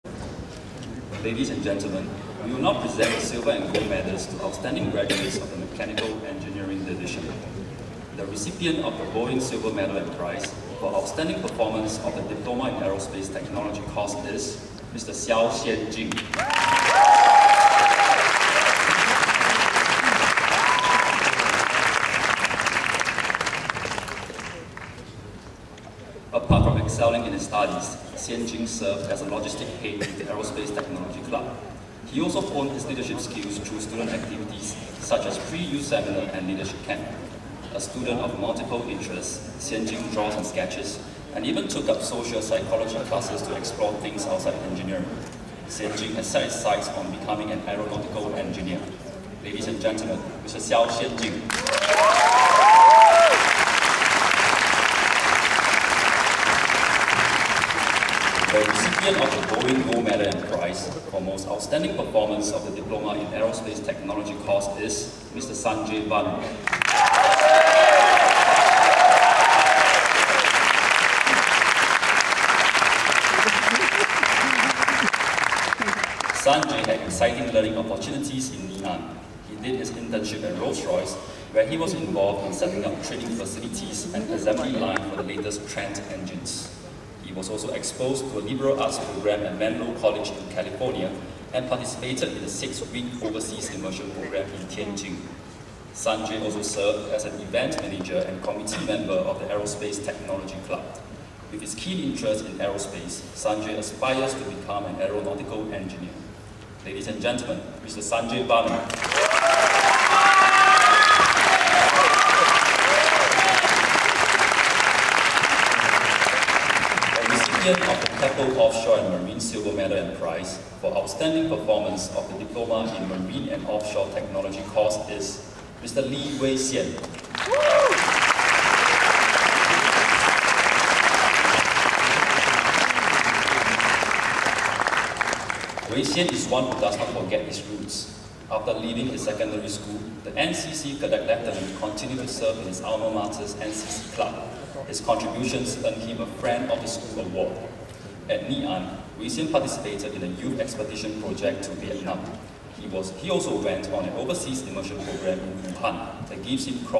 Sanjay Vanen ngee ann 2012 silver and gold medalist_1.MPG
Part 1 :The 6 Gold and Silver medalists from Ngee Ann Polytechnic Mechanical Engineering Department. Sanjay Vanen receiving the Gold Medal  for the Most Outstanding Performance in Aerospace Technology
In order of appearance:
Seow Xian Jin
Sanjay Vanen
Lee Wei Xian
Shawn Ng Jun Hwee
John Chua Mu En
Eugene Yeo Wei